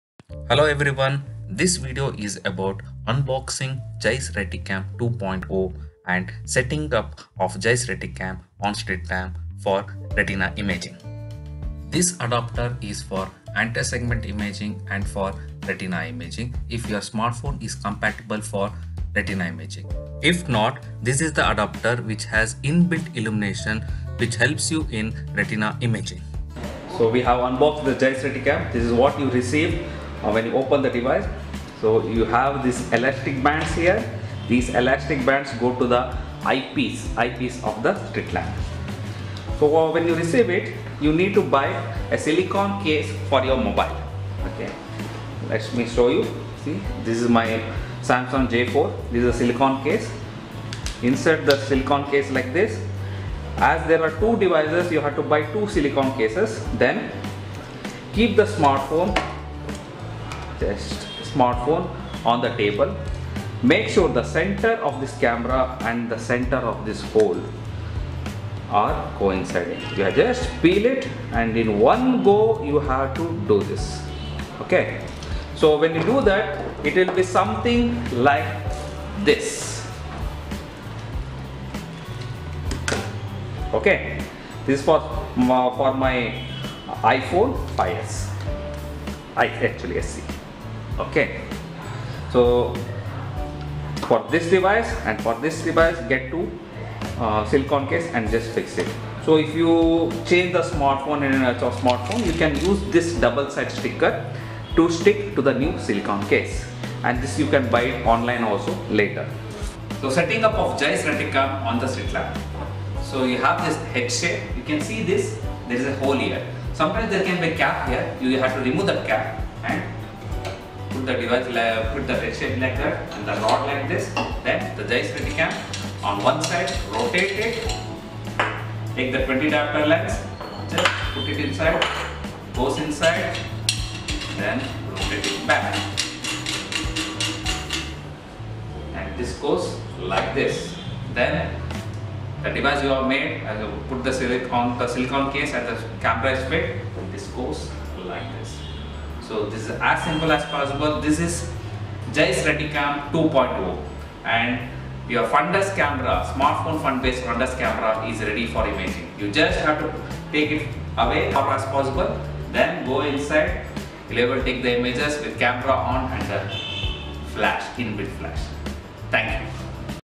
Unboxing Jaiz RetiCAM 2.0 - Smartphone based Fundus Camera
Jaiz RetiCAM 2.0 - Fundus Camera (Smartphone based)
It's slitlamp mounted for stable imaging.
Compatible for iPhone, Android Device and Smartphone with multiple camera.

Device is basically designed for Volk 20D lens.
20D lens is not included with the device.
Any query please let us know.

An Innovation by Dr Jaitra

For more details and to buy the device WApp to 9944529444